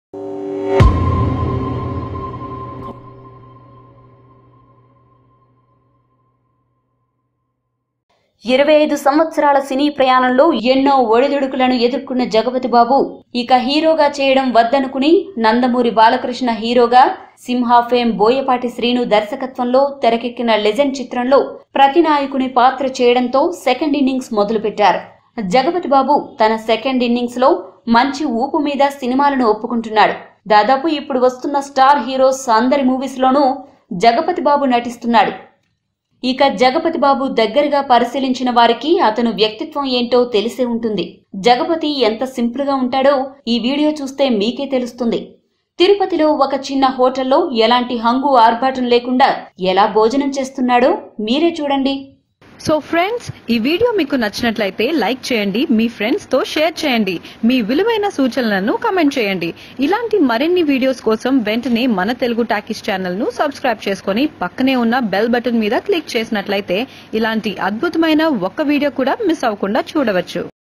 తిరుపతిలో ఒక చిన్నహోటల్లో భోజనం చేస్తూ కనిపించిన మన కూల్ హీరో ఎవరో తెలిస్తే షాక్ అవుతారు
తిరుపతిలో ఒక చిన్నహోటల్లో భోజనం చేస్తూ కనిపించిన మన కూల్ హీరో ఎవరో తెలిస్తే షాక్ అవుతారు || Telugu Talkies
25 సంత్సరాల సినీ ప్రయాణంలో ఎన్నో ఒడిదొడుకులను ఎదుర్కున్న జగపతిబాబు, ఇక హీరోగా చేయడం వద్దనుకొని నందమూరి బాలకృష్ణ హీరోగా సింహా ఫేం బోయపాటి శ్రీనుదర్శకత్వంలో తెరకెక్కిన లెజెండ్‌ చిత్రంలో ప్రతినాయకుని పాత్ర చేయడం సెకండ్ ఇన్నింగ్స్ మొదలుపెట్టారు.
జగపతి బాబు తన సెకండ్ ఇన్నింగ్స్ లో మంచి ఊపు మీద సినిమాలని ఒప్పుకుంటున్నాడు. దాదాపు ఇప్పుడు వస్తున్న స్టార్ హీరోస్ అందరి మూవీస్ లోను జగపతి బాబు నటిస్తున్నాడు.